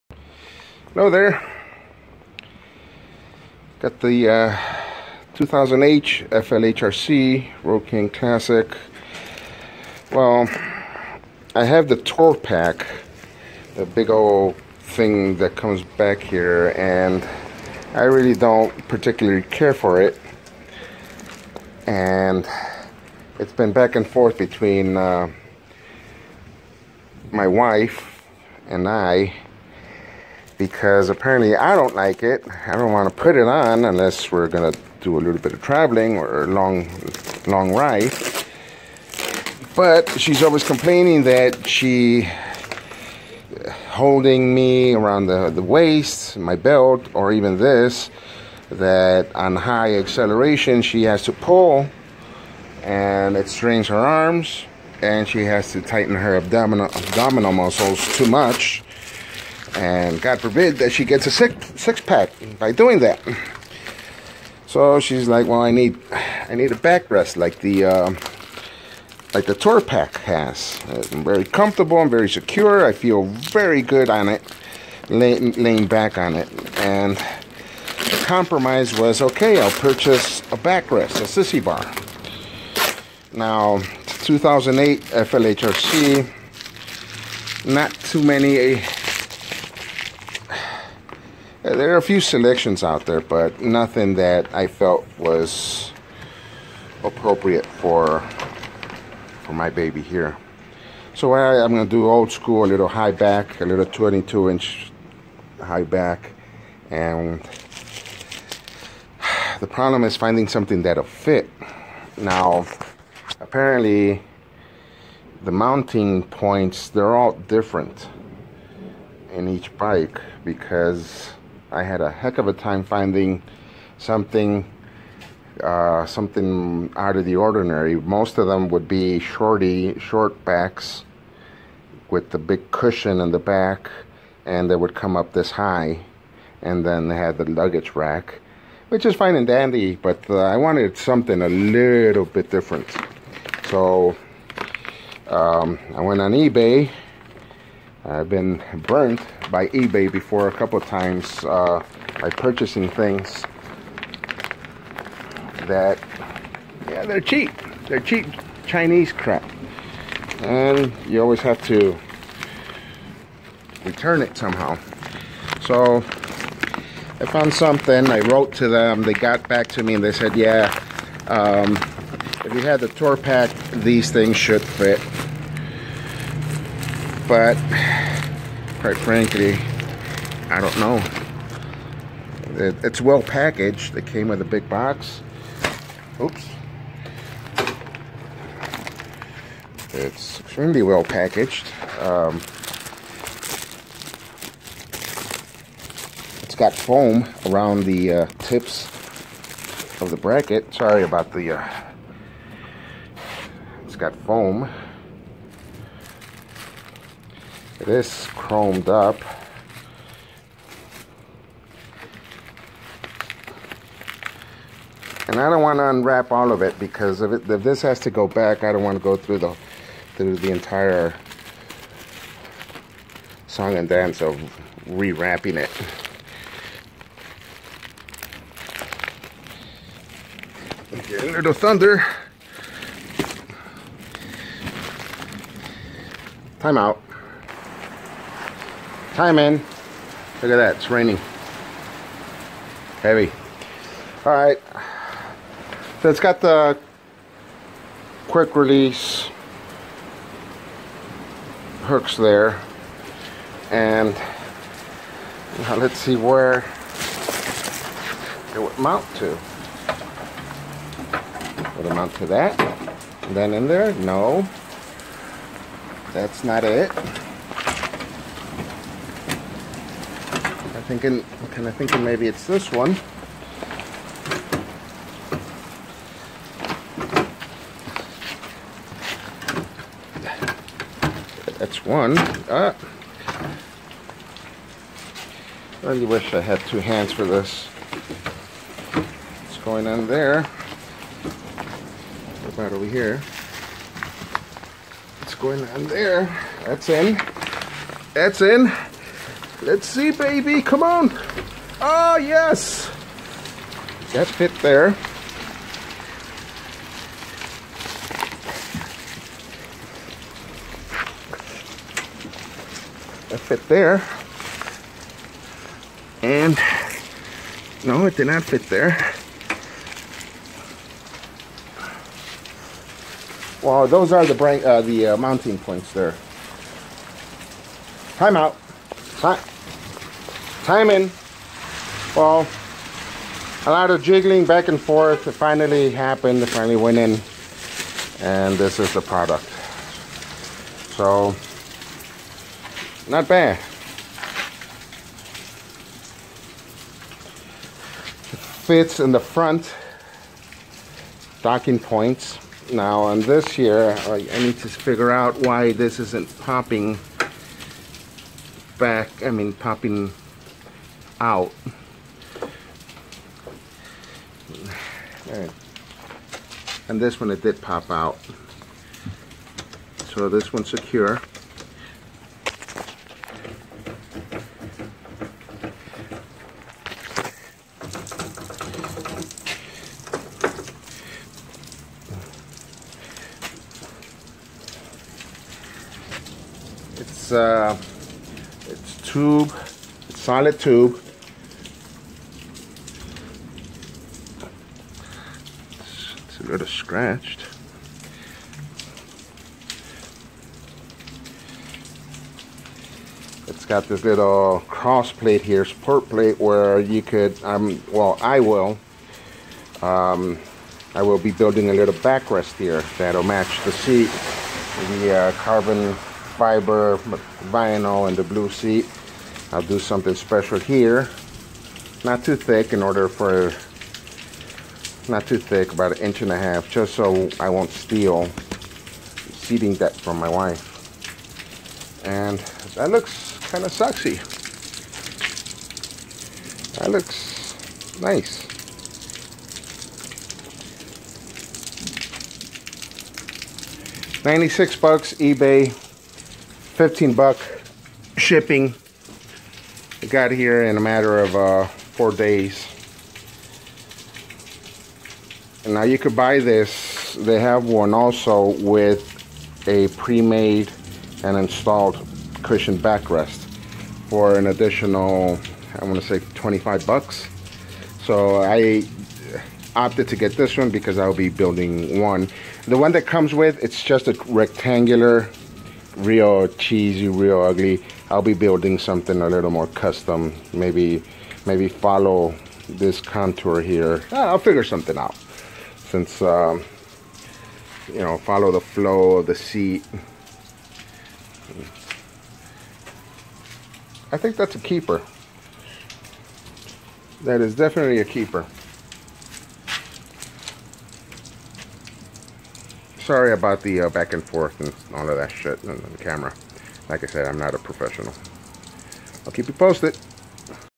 Sexy sissy bar for 08 Road King, ebay inexpensive good quality back rest.
description and install of harley sissy bar back rest.